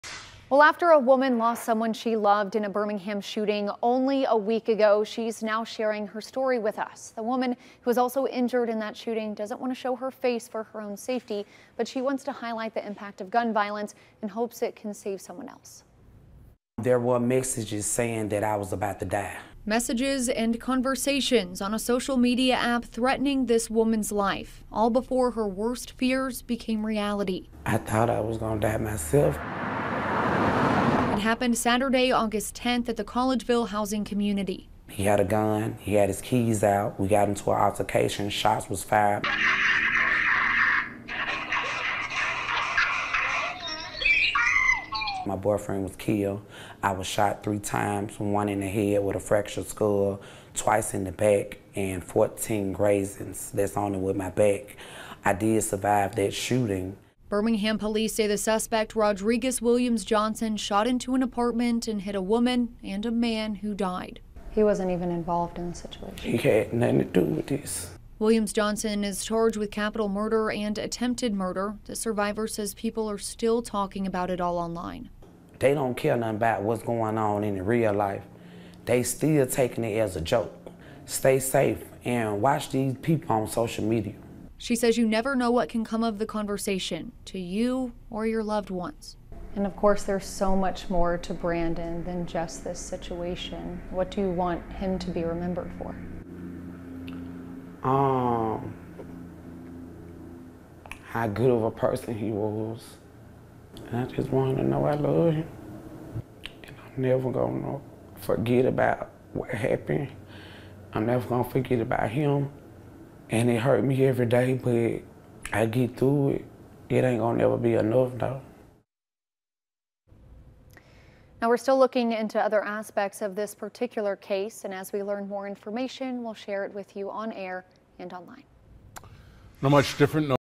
Birmingham shooting survivor mourning boyfriend killed in same attack
"I just want him to know I love him"

For more news from central Alabama, visit WBRC.com